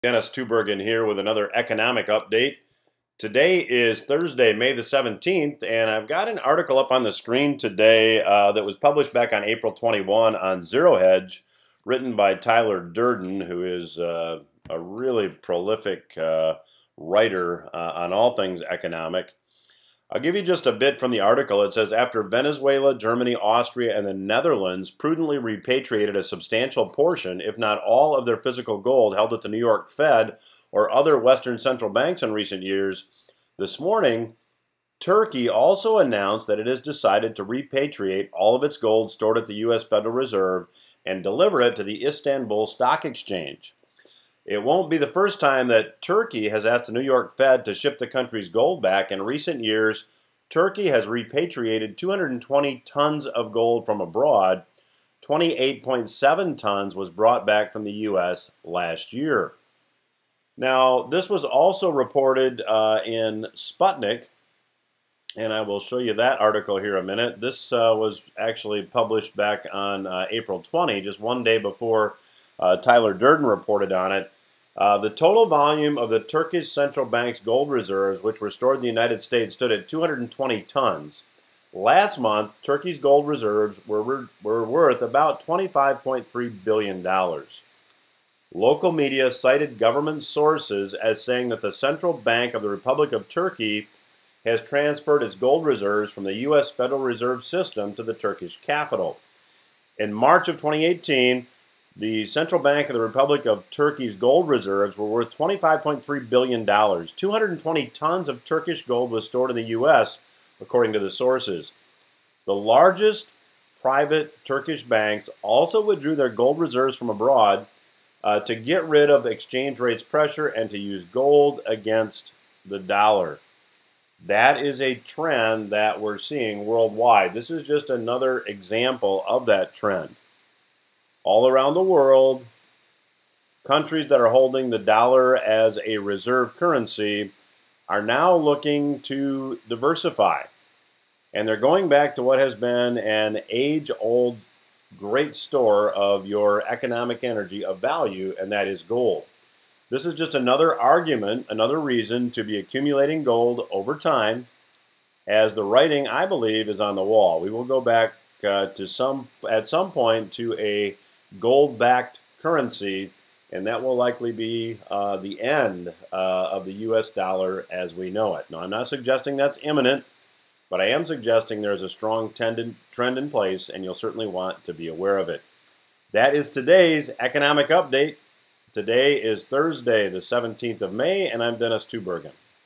Dennis Tubbergen - Economic Update 18-05-17
On today’s economic update, Dennis Tubbergen discusses:

-the growing movement of gold repatriation

-why countries are moving gold out of the US

-what you should be doing now
Dennis Tubbergen comments on the broad financial markets.

Delivered every Monday evening via e-mail, it contains some of the most insightful and accurate market and economic commentary and analysis available anywhere.